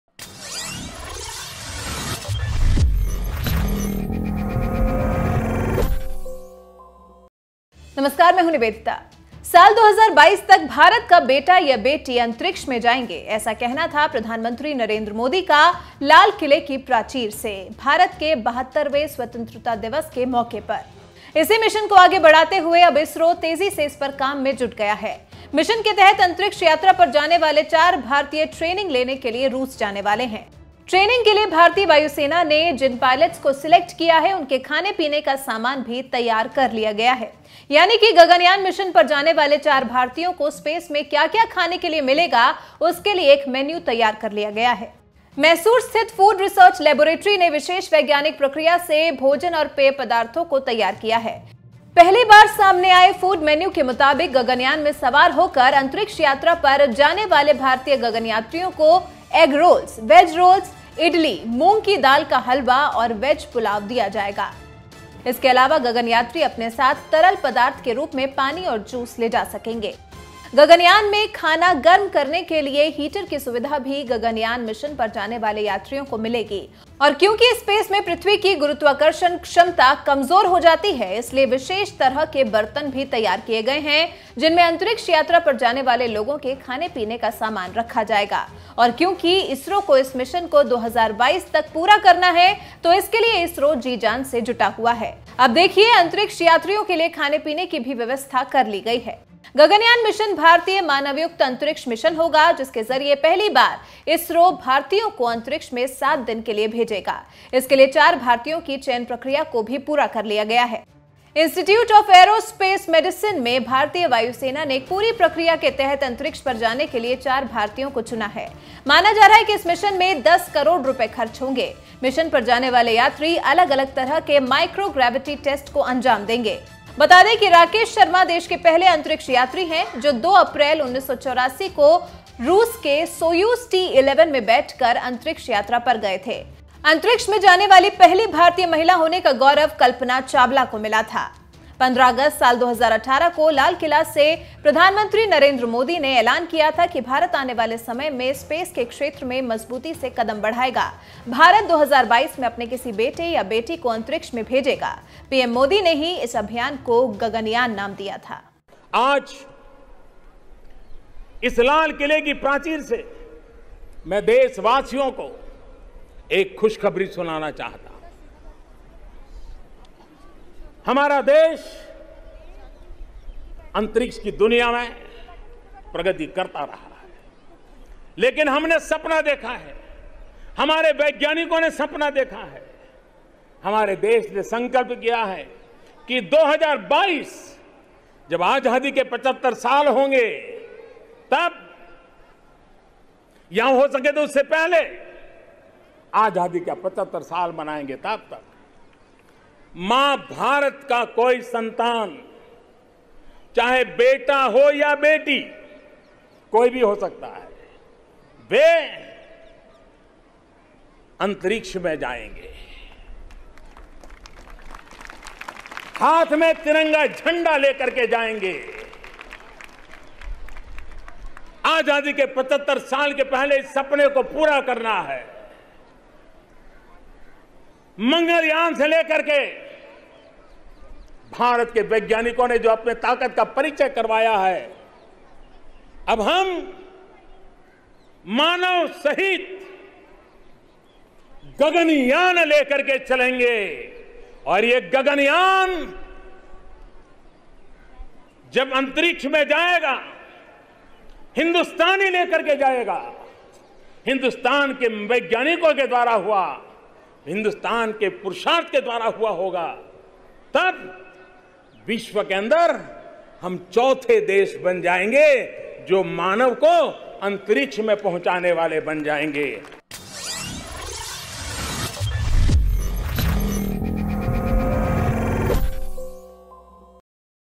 Gaganyaan पर जाने वाले यात्रियों का जबरदस्त Menu देख आपकी जीभ लपलपा जाएगी !
मिशन गगनयान के तहत अंतरिक्ष में जाने वाले यात्रियों के लिए मेन्यू तैयार कर लिया गया है…तैयार स्वादिष्ट मेन्यू को देखकर आपके मुंह में भी पानी आ जाएगा…रिपोर्ट में देखिए क्या खास व्यंजनों का लुत्फ उठा पाएंगे भारत से जाने वाले अंतिरक्ष यात्री ??